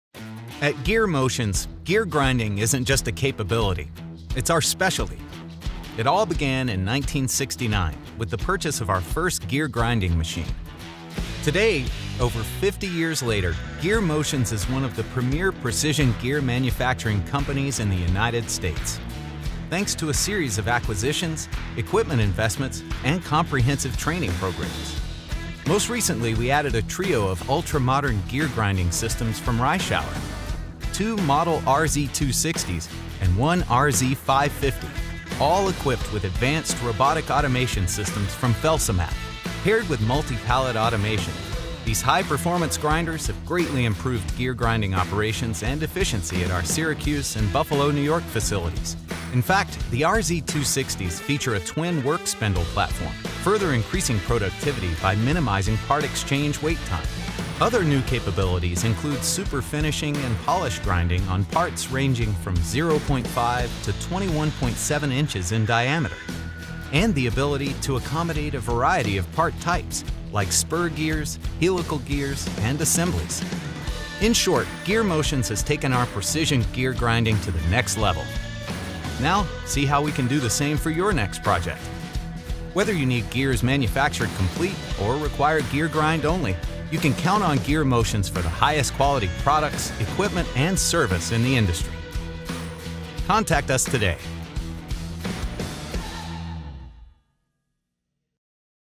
Gear Motions: Gear Grinding
At Gear Motions, gear grinding isn’t just a capability – it’s our specialty.

It all began in 1969, with the purchase of our first gear grinding machine.
Today, over 50 years later, Gear Motions is one of the premier precision gear manufacturing companies in the United States, thanks to a series of acquisitions, equipment investments and comprehensive training programs.

Most recently, we added a trio of ultramodern gear grinding systems from Reishauer, two model RZ260s and one RZ550, all equipped with advanced robotic automation systems from Felsomat.

Paired with multi-pallet automation, these high-performance grinders have greatly improved gear grinding operations and efficiency at our Syracuse and Buffalo, New York facilities.

In fact, the RZ260s feature a twin work spindle platform – further increasing productivity by minimizing part exchange wait time.

Other new capabilities include super finishing and polish grinding on parts ranging from 0.5 to 21.7 inches in diameter and the ability to accommodate a variety of part types, like spur gears, helical gears and assemblies.

In short, Gear Motions has taken our precision gear grinding to the next level. Now, see how we can do the same for your next project.

Whether you need gears manufactured complete, or require gear grind only, you can count on Gear Motions for the highest-quality products, equipment and service in the industry.